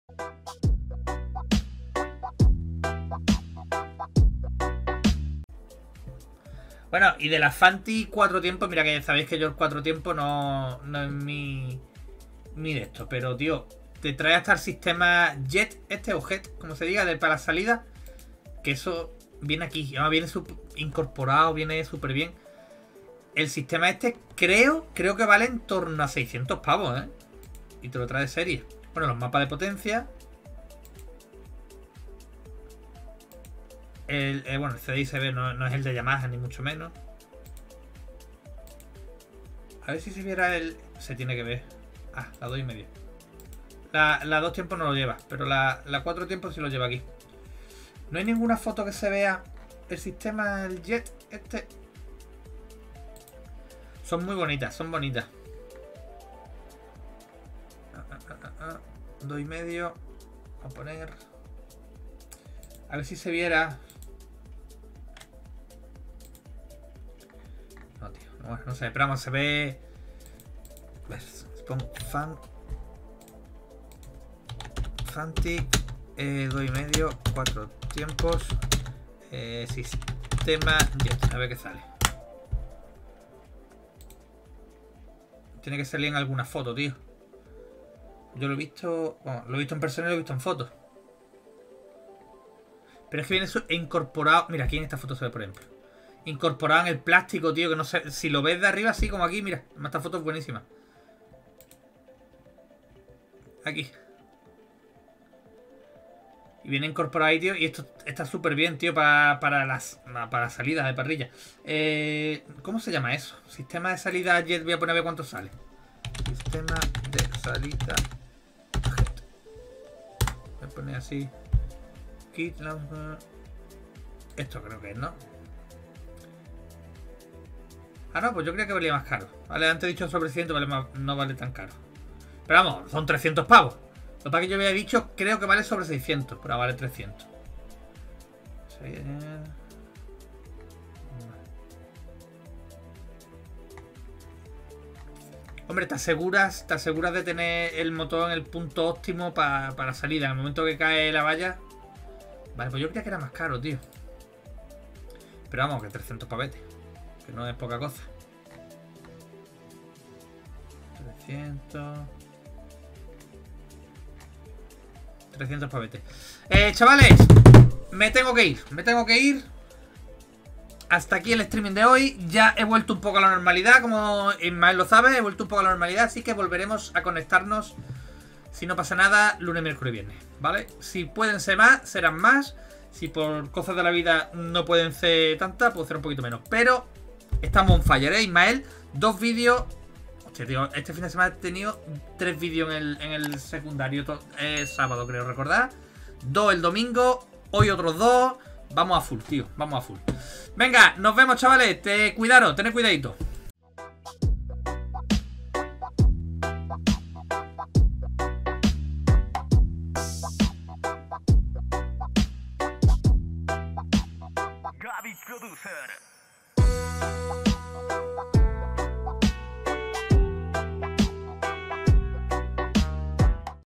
Indicador REVOLUCIONES de Salida GET para las FANTIX XXF de serie🤯​
Muy buenas amigos!!! En este video comentamos un poco sobre las FANTIC después de leer la noticia de Escandell y su fichaje por la marca, personalmente es una moto que me llama muchisimo la atención y creo que tiene detalles de calidad que son impresionantes...